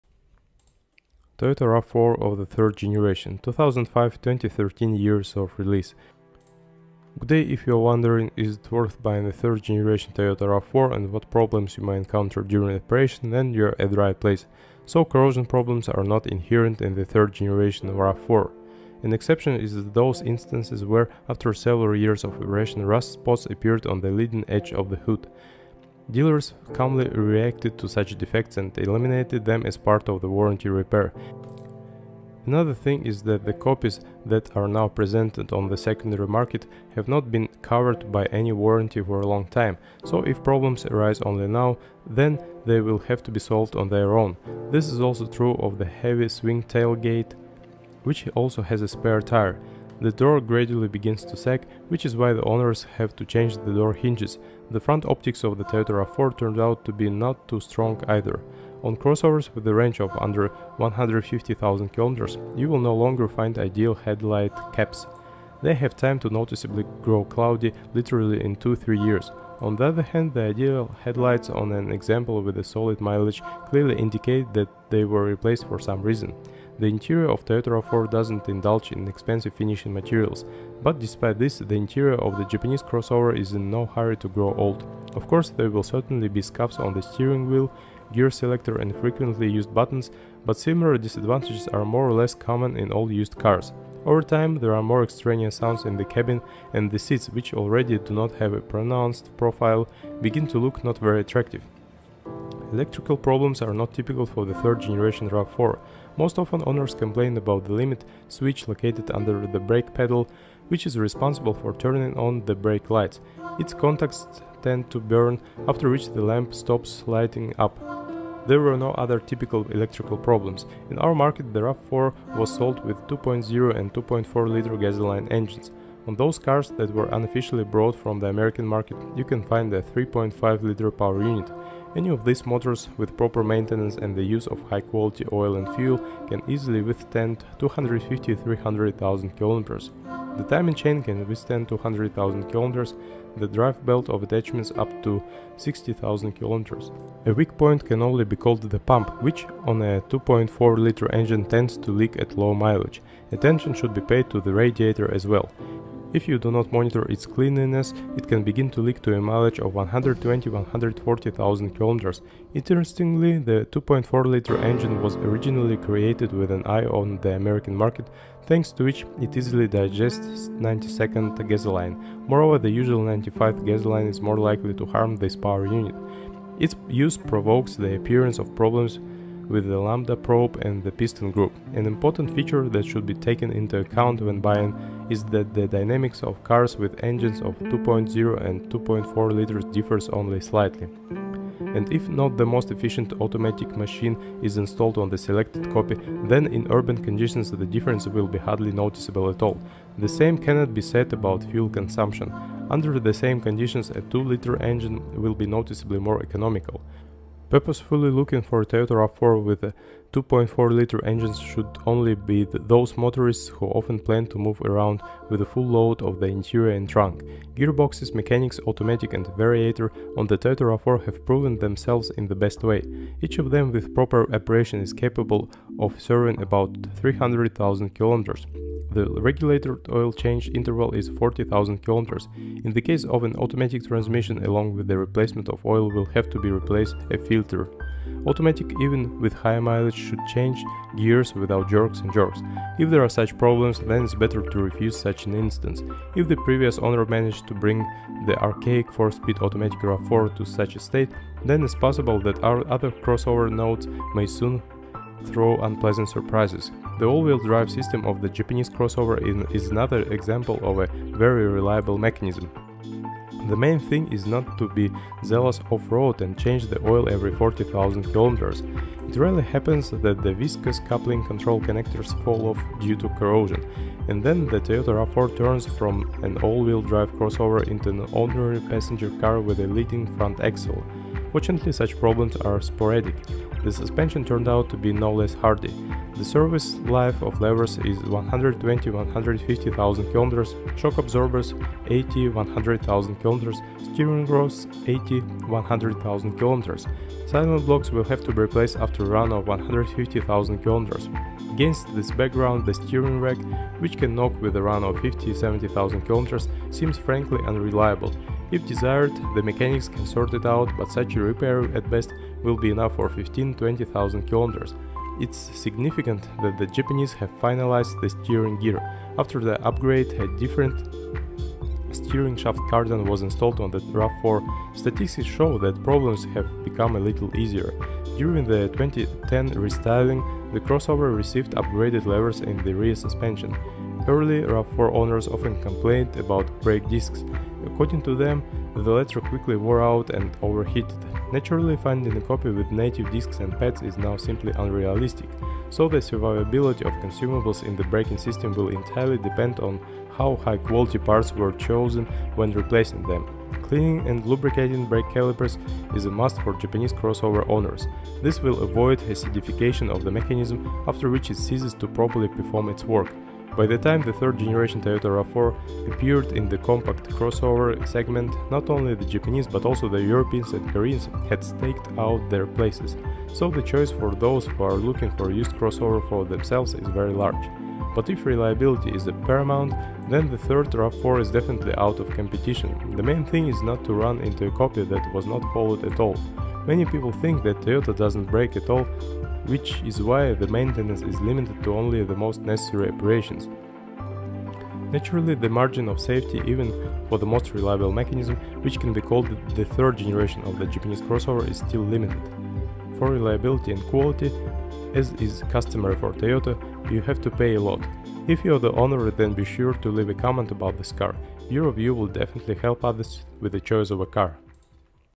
Why did I sell Toyota RAV4 3? Cons of used RAV4 III with mileage
More details about the pitfalls when buying "Toyota RAV4". Why and for what reasons they can sell or simply "get rid" of a used car called Toyota RAV4 of the third generation (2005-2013 years of release). We are talking about the problems and weaknesses of a used car.